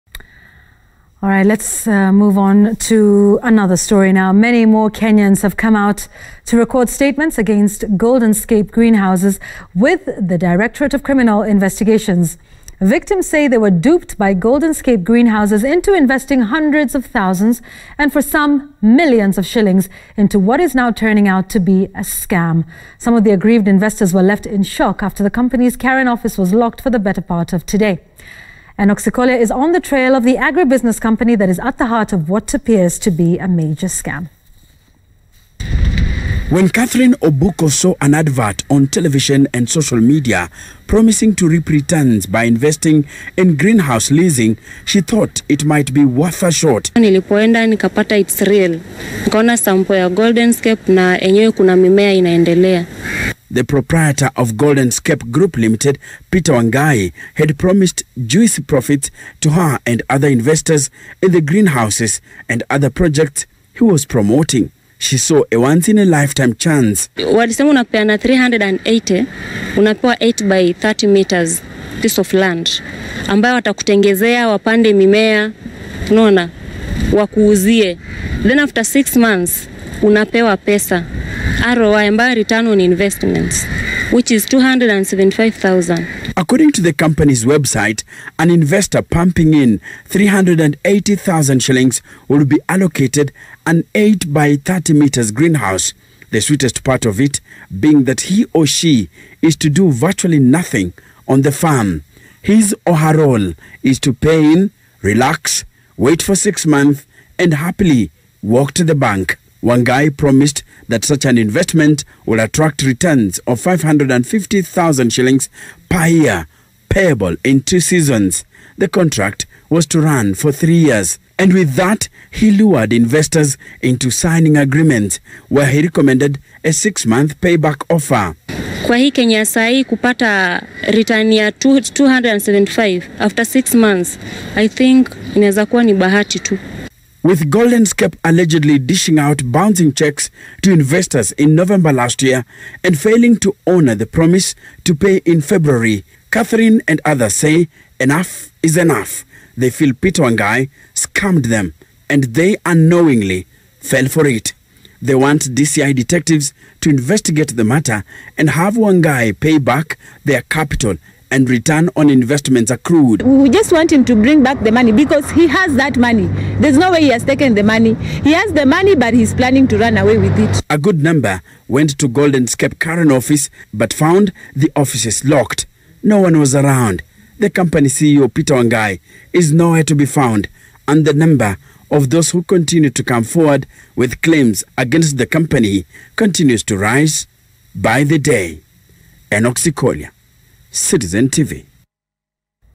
More investors present complaints to DCI investigators over Goldenscape scam
Many more Kenyans have come out to record statements against goldenscape greenhouses with the directorate of criminal investigations.
Victims say they were duped by goldenscape greenhouses into investing hundreds of thousands and for some millions of shillings into what is turning out to be a scam.
Some of the aggrieved investors were left in shock after the company's Karen office was locked for the better part of the today.